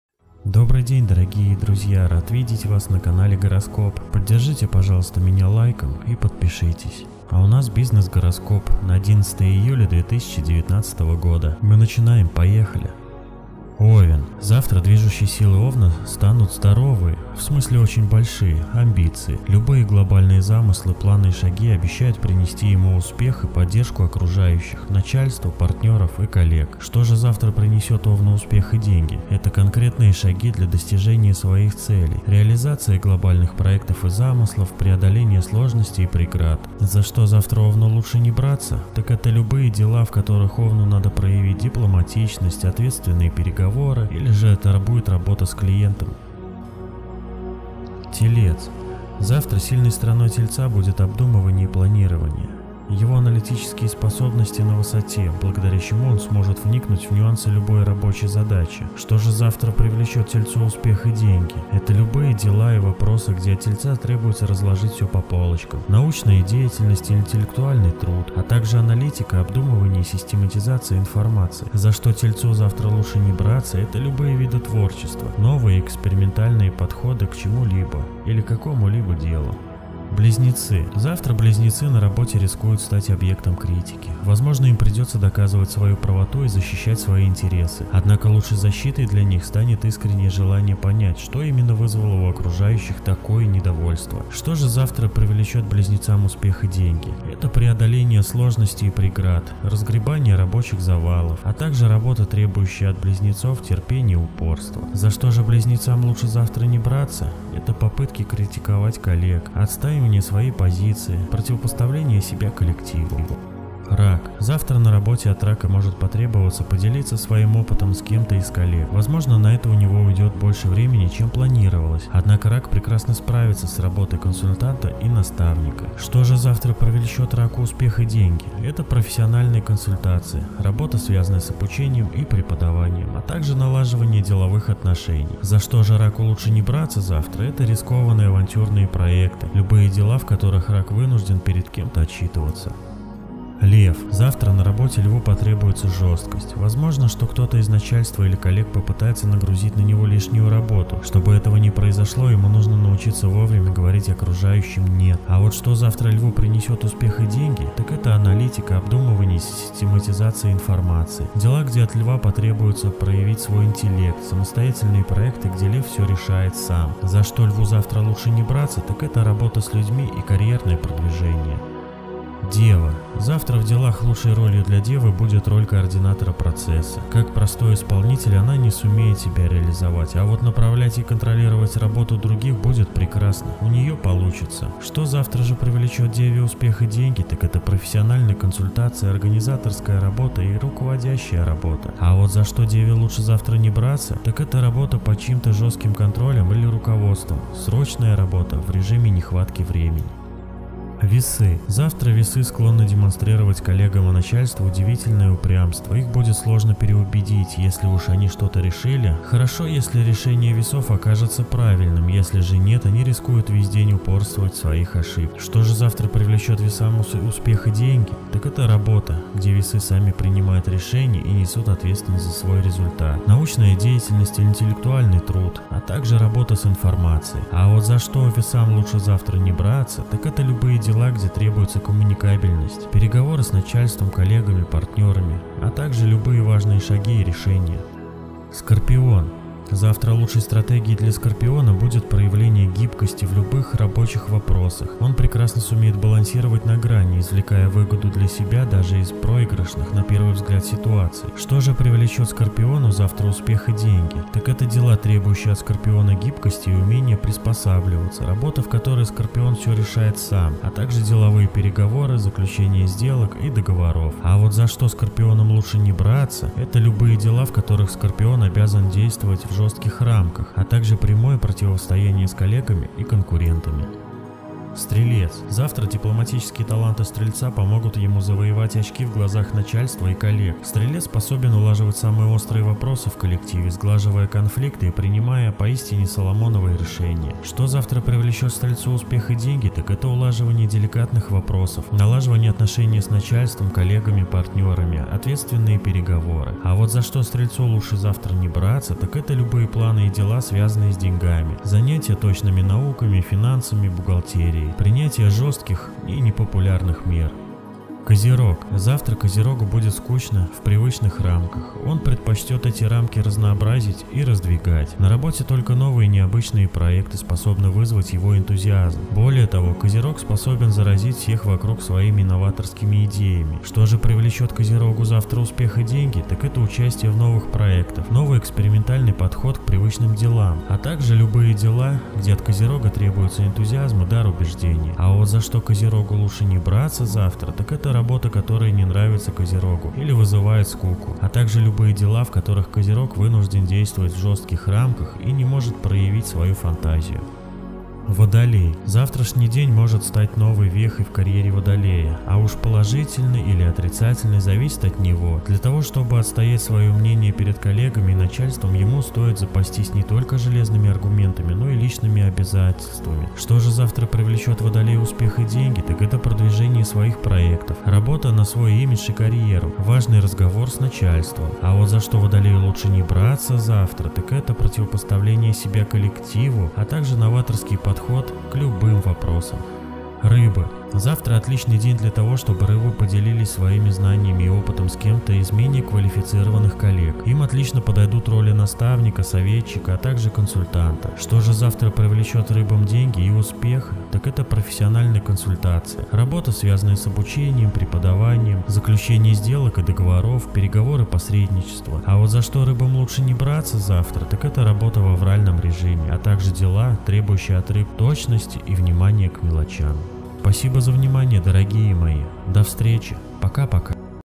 БИЗНЕС ГОРОСКОП НА 11 ИЮЛЯ 2019 ГОДА / ЕЖЕДНЕВНЫЙ ГОРОСКОП
Отличного вам дня, дорогие друзья!

 Бизнес гороскоп на сегодня
 Овен
 Телец
 Близнецы
 Рак
 Лев
 Дева
 Весы
 Скорпион
 Стрелец
 Козерог
 Водолей
 Рыбы.